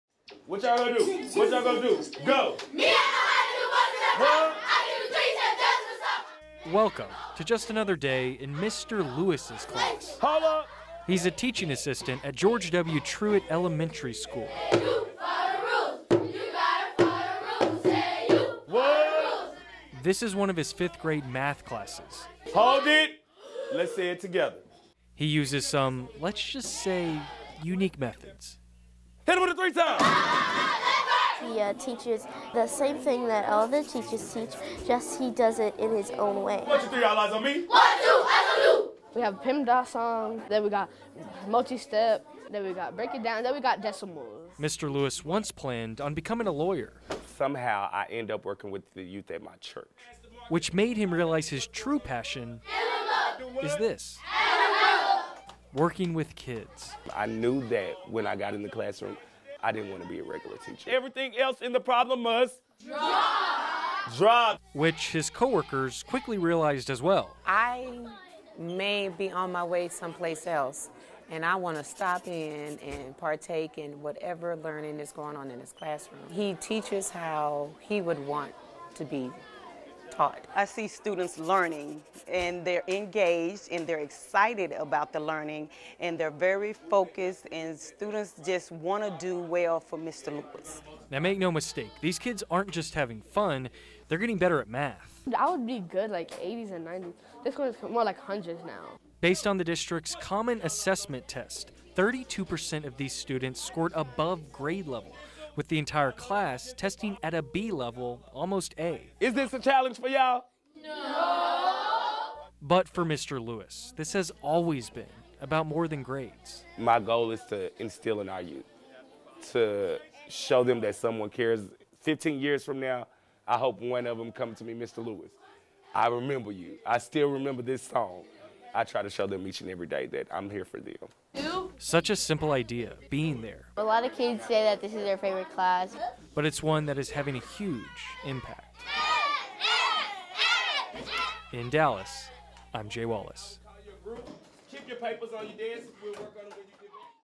Dallas teacher using rap to teach math
Mr. Lewis teaches math with music -- with rap. And while it might initially just look like fun, these kids are using what they're learning like never before.